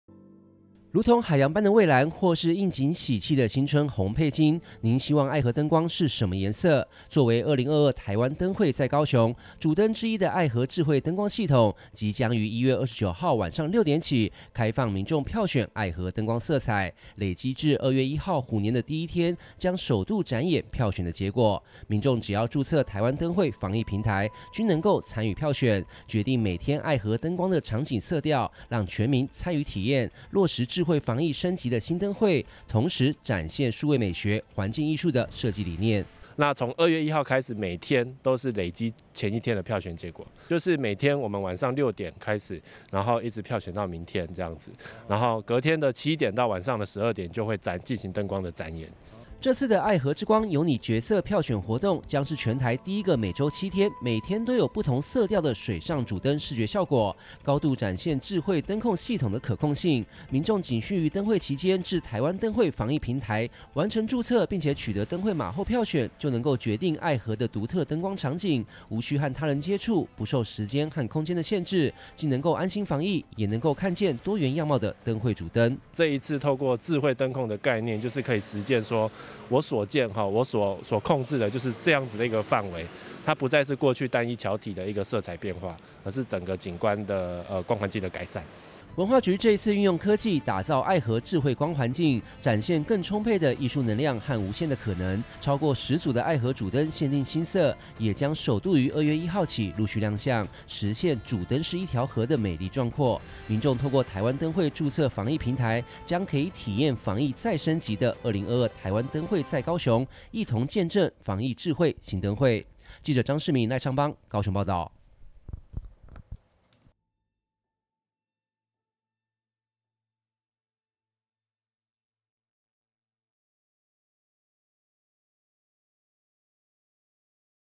票選決色|台灣燈會再創紀錄 愛河之光由你決色
台灣燈會再創紀錄 愛河之光由你決色
 您希望今天的愛河燈光是什麼顏色？作為「2022台灣燈會在高雄」主燈之一的愛河智慧燈光系統，繼全台最大聖誕主燈，展現絕美燈光後再創紀錄，即將於1月29日晚間18時起，開放民眾票選愛河燈光色彩，讓全民參與體驗一場智慧升級的新燈會，展現數位美學，愛河光環境藝術的設計理念。